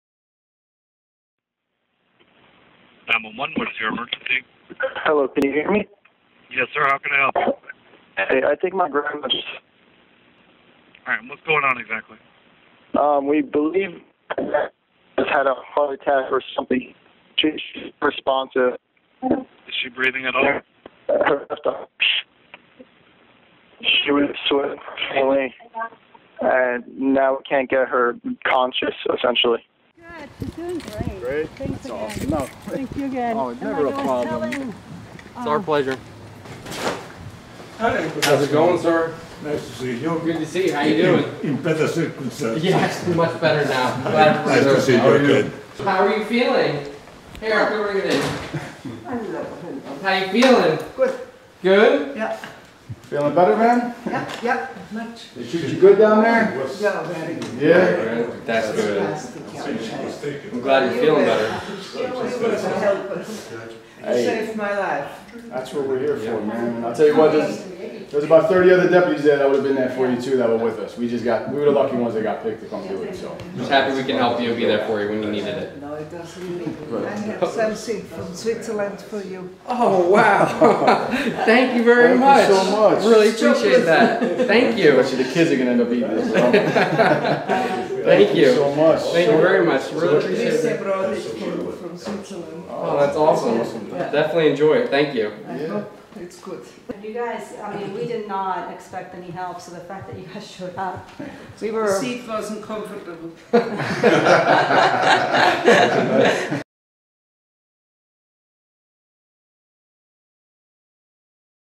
To Serve and Protect. Period.
It was a family's nightmare: Midnight. Hurricane Irma raging outside. An elderly grandmother unconscious.

They called 9-1-1 only to learn the storm's ferocity prevented emergency crews from responding. They called again. Sheriff's Major Kristi Poore listened and acted. She directed deputies to check the weather, and if they thought it was safe enough, to go rescue the grandmother. Deputies Daniel Heffron and Anthony Forte didn't hesitate. They headed out in a patrol car to the home of 83-year-old Rosemary Dunner.

She was breathing, but in dire need of medical attention. The deputies, with Rosemary and her daughter in the patrol car, made their way in driving wind and rain, along deserted roads, to St. Joseph's Hospital North.

Rosemay spent a couple of days in the hospital, before she returned home thankful and grateful. At a reunion with the deputies who rescued her that frightful night, she gave them hugs and boxes of chocolate from her native Switzerland.